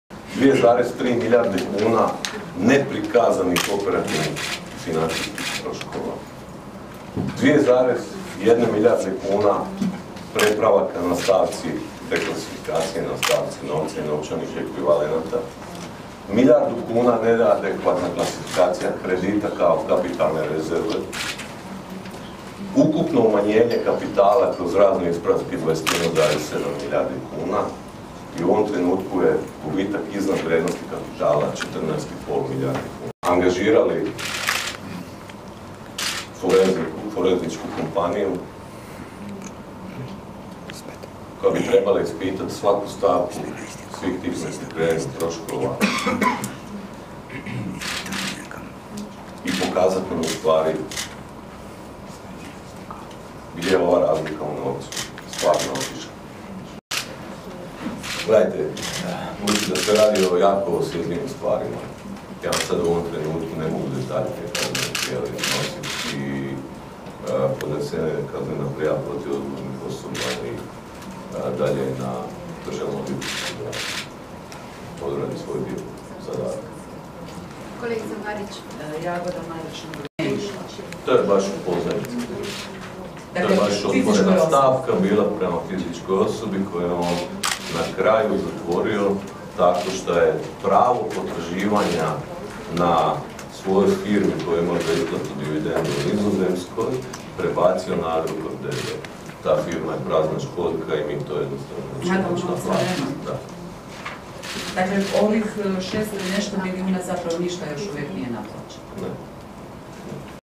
Predstavljeni najvažniji nalazi revidiranog izvješća Agrokora i financijski izvještaj grupe
Zagreb, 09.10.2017 - Izvanredna uprava Agrokora nakon sjednice Privremenog vjerovničkog vijeća na konferenciji za medije predstavila je najvažnije nalaze revidiranog financijskog izvješća Agrokora d.d. kao i revidirani konsolidirani financijski izvještaj grupe. Na fotografiji Vladin izvanredni povjerenik za Agrokor Ante Ramljak. Video HINA/ Denis CERIĆ /ml